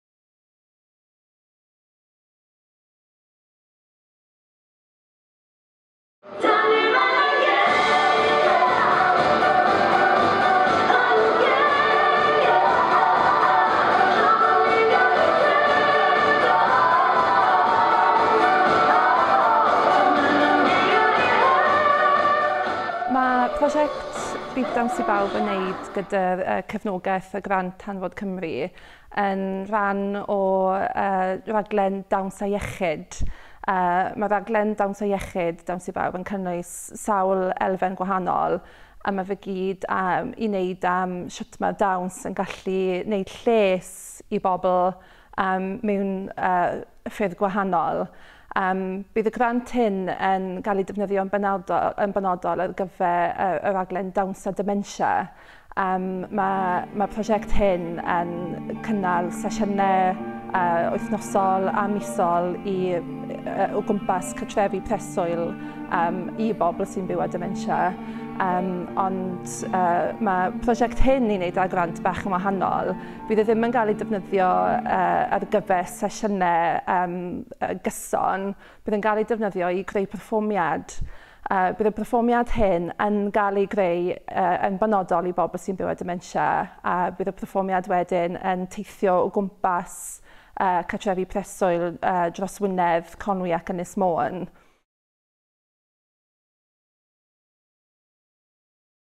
Dawns i Bawb
Grŵp dawns cymunedol yw Dawns i Bawb sy’n ceisio cynnal darpariaeth dawns drwy gyfrwng y Gymraeg. Mae’n bwriadu defnyddio’r grant oddi wrth Hanfod Cymru er mwyn datblygu ei rhaglen ddawns Dementia yn dilyn llwyddiant ei raglen dawns mewn cartrefi preswyl i'r henoed yng Ngwynedd, Conwy ac Ynys Môn. Mae’r fenter yn dilyn adroddiadau o sawl astudiaeth ymchwil sydd wedi profi y gall y celfyddydau fod o fudd i bobl sy’n byw gyda Dementia.